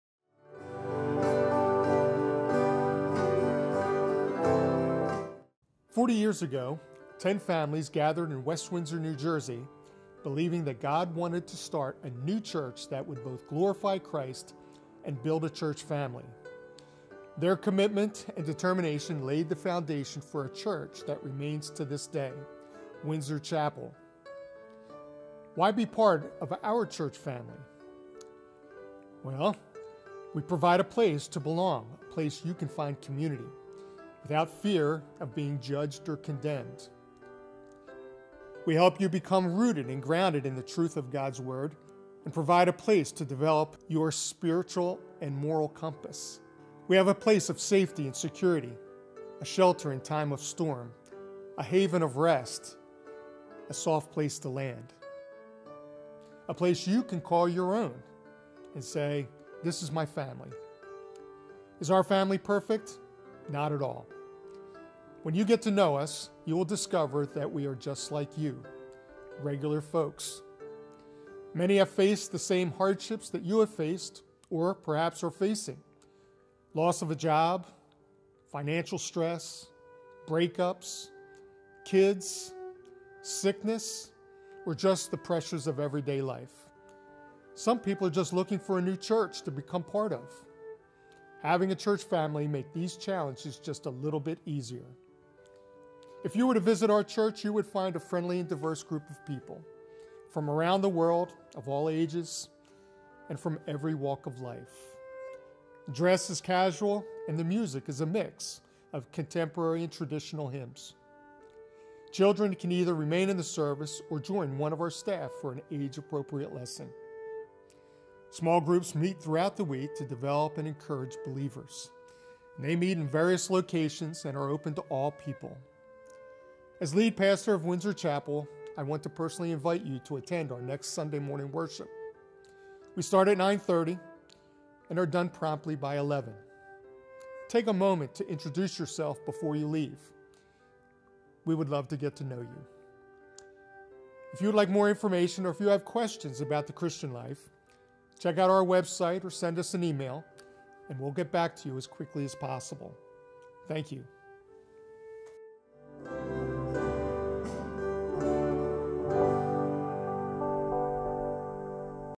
Welcome to Windsor Chapel
Watch this short video to learn more about Windsor Chapel. Windsor Chapel sits in the middle of a community of thousands of families from around the globe.  It is our desire to serve our community with the love and care of Jesus and to provide a safe and welcoming environment in the midst of the very active, often stressful day-to-day of life.  We are committed to:

* Growing in a genuine and intimate relationship with the Lord – Matthew 22:37

* Learning and applying the transforming truths of God’s Word – Romans 12:1, 2; Ephesians 2:10

* Developing relationships that reflect the love and care of Jesus, where we actively serve God and others through our time, treasure and talent – 1 Peter 4:10

* Sharing the message of love, hope and forgiveness found in Jesus with our friends, our community and the world – Matthew 28:18-20